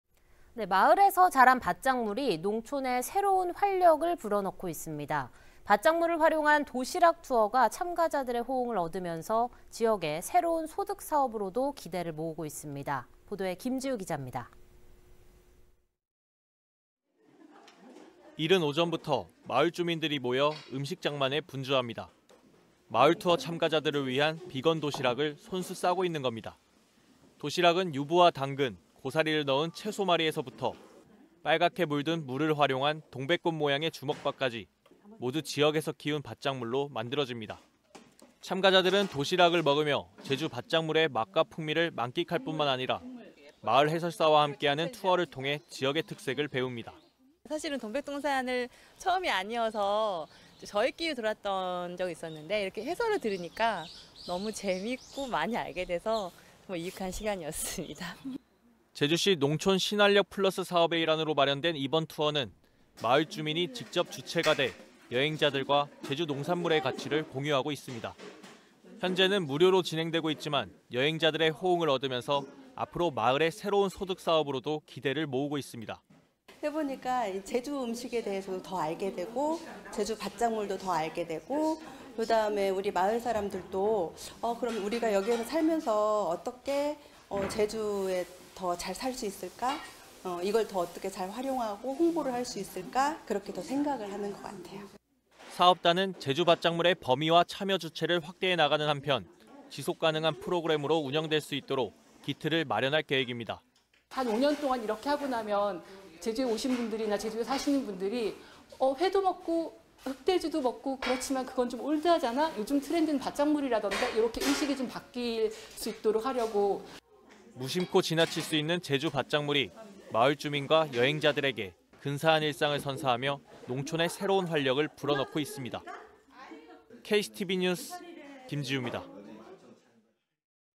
제주 밭작물이 선사하는 근사한 일상
마을에서 자란 밭작물이 농촌에 새로운 활력을 불어넣고 있습니다.

밭작물을 활용한 도시락 투어가 참가자들의 호응을 얻으면서 앞으로 지역의 새로운 소득사업으로도 기대를 모으고 있습니다.